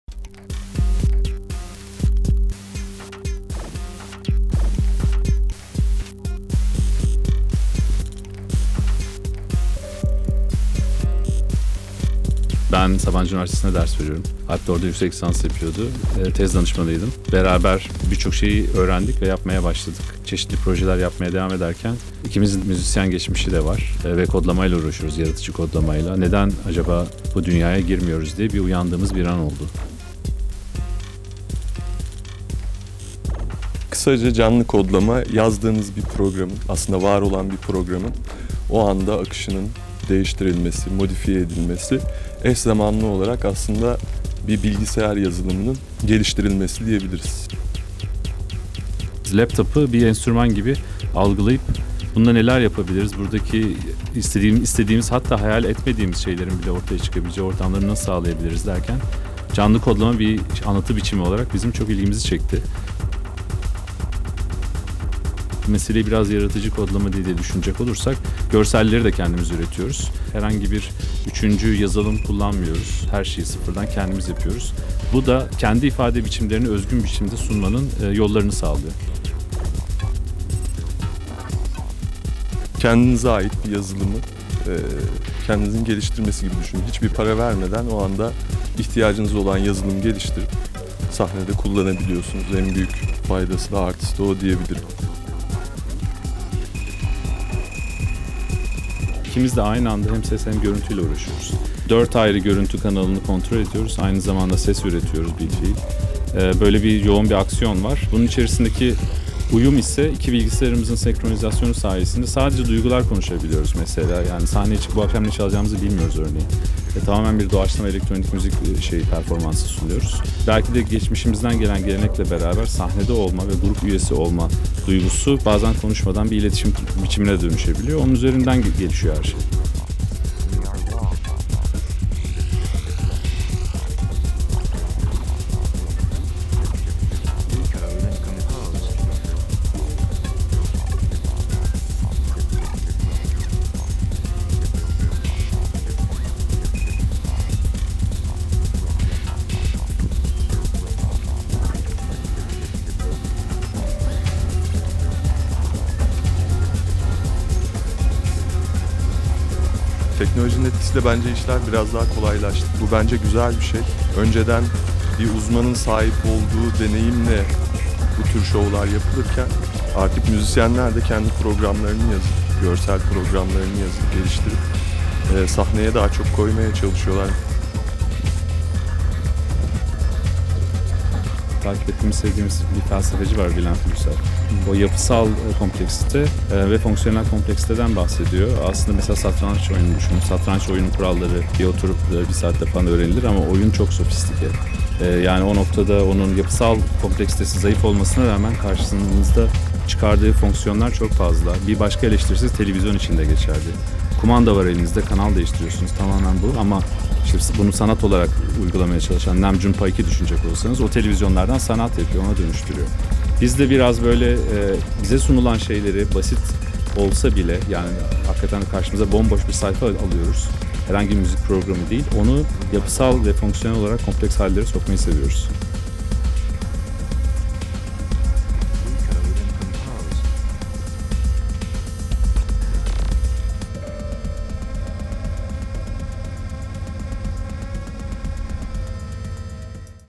Behind The Scenes: R.A.W Live Coding
RAW yaratıcılığı kendine özgü yöntemlerle ifade etmek için yola çıkmış bir ses ve görüntü grubudur. Kullandıkları ses ve görüntü ham maddelerini eş zamanlı olarak programlayarak (live-coding) canlı performansa dönüştüren ekip Selçuk ARTUT (Replikas) ve Alp TUĞAN'dan oluşmaktadır.

Bu performans Sonar İstanbul 2020'de kaydedilmiştir.

Yeni medya sanatlarının öncü çalışmalarını Digilogue ile keşfet!